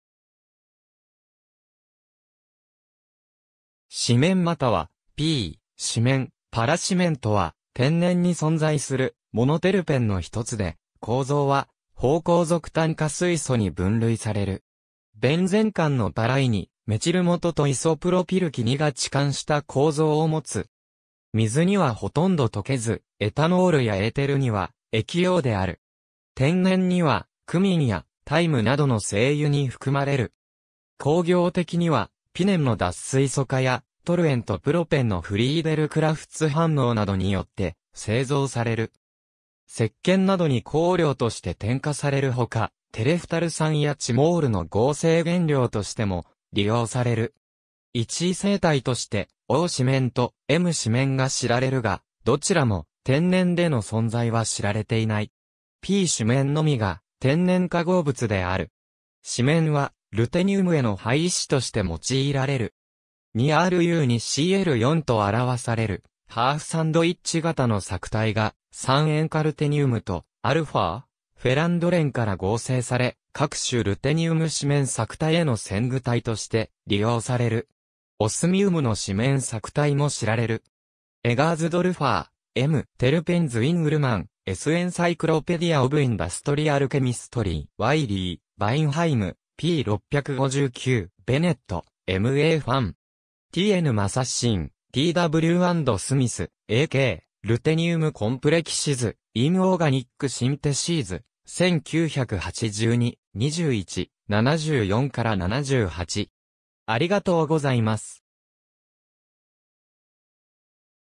シメン
シメン (cymene) またはp-シメン、パラシメン (p-cymene) とは、天然に存在するモノテルペンのひとつで、構造は芳香族炭化水素に分類される。
ベンゼン環のパラ位（1位と4位）にメチル基 (-CH3) とイソプロピル基 (-CH(CH3)2) が置換した構造を持つ。
水にはほとんど溶けず、エタノールやエーテルには易溶である。
天然にはクミンやタイムなどの精油に含まれる。
工業的には、ピネンの脱水素化や、トルエンとプロペンのフリーデル・クラフツ反応などによって製造される。
石鹸などに香料として添加されるほか、テレフタル酸やチモールの合成原料としても利用される。
位置異性体として o-シメン（CAS登録番号 [527-84-4]）と m-シメン（同 [535-77-3]）が知られるが、どちらも天然での存在は知られていない。
p-シメンのみが天然化合物である。
シメンはルテニウムへの配位子として用いられる。
(η6-cymene)2Ru2Cl4 と表されるハーフサンドイッチ型の錯体（CAS 登録番号 [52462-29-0]）が、三塩化ルテニウムと α-フェランドレンから合成され、各種ルテニウムシメン錯体への前駆体として利用される。
オスミウムのシメン錯体も知られる（例: (η6-cymene)2Os2Cl4、CAS 登録番号 [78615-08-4]）。
^ Eggersdorfer, M. (2003). "Terpenes" in Ullmann's Encyclopedia of Industrial Chemistry. Wiley: Weinheim, p. 659. ^ Bennett, M. A.; Huang,
T. N.; Matheson, T. W. and Smith, A. K. "(h6-Hexamethylbenzene)ruthenium complexes", Inorganic Syntheses, 1982, 21, 74-78.